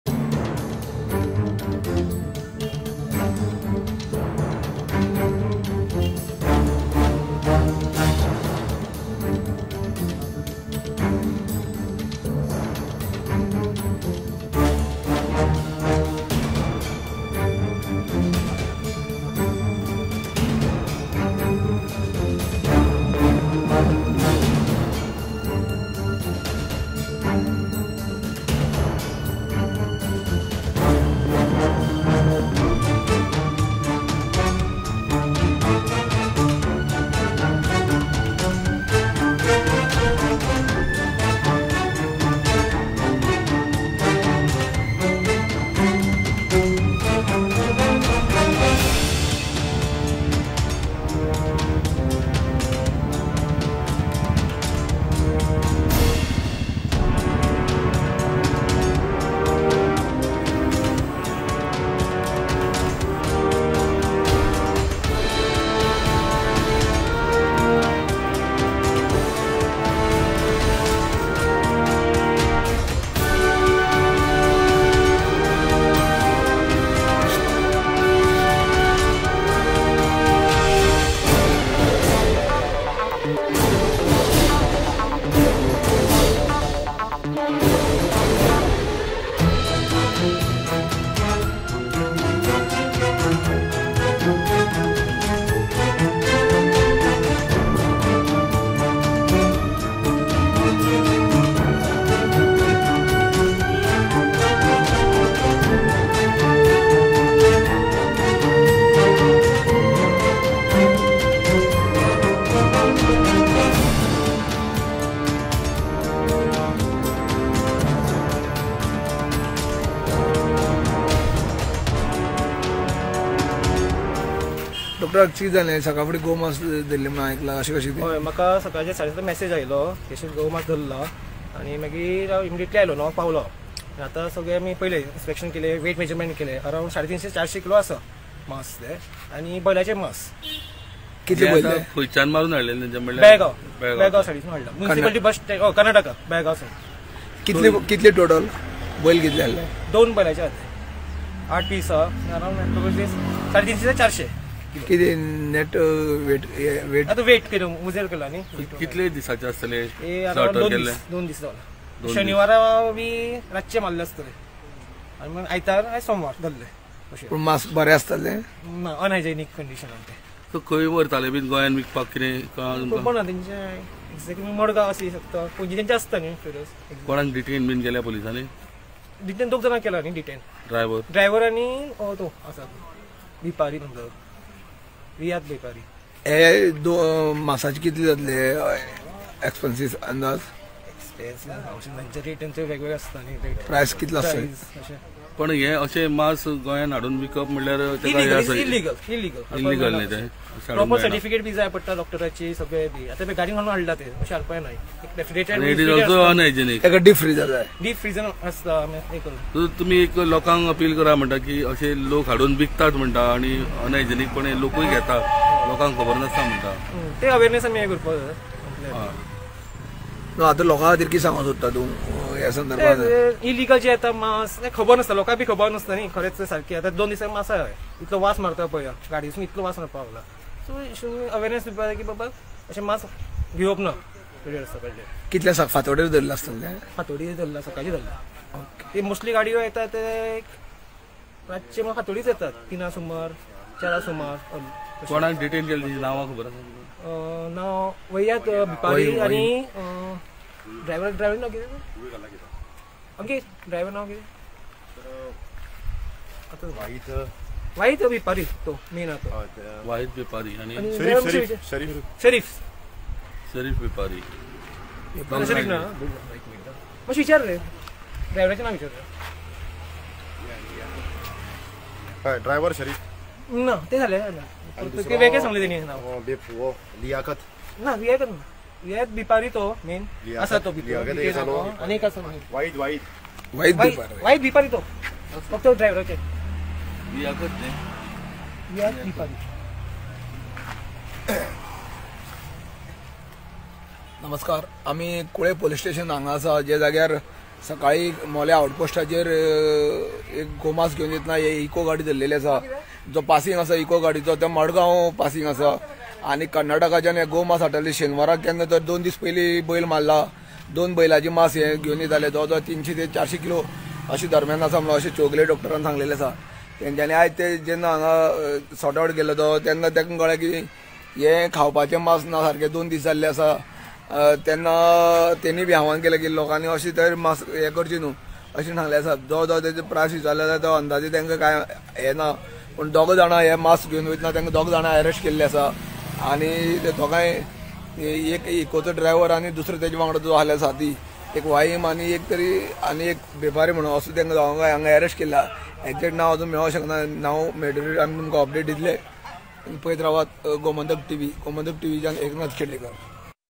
Molem Illegal Beef Smuggling - कर्नाटकातून गोव्यात गोमांसाची तस्करी; मोले चेकपोस्टवर संशयित ताब्यात
Illegal Beef Smuggling - कर्नाटकातून गोव्यात गोमांसाची तस्करी; मोले चेकपोस्टवर संशयित ताब्यात | Gomantak Tv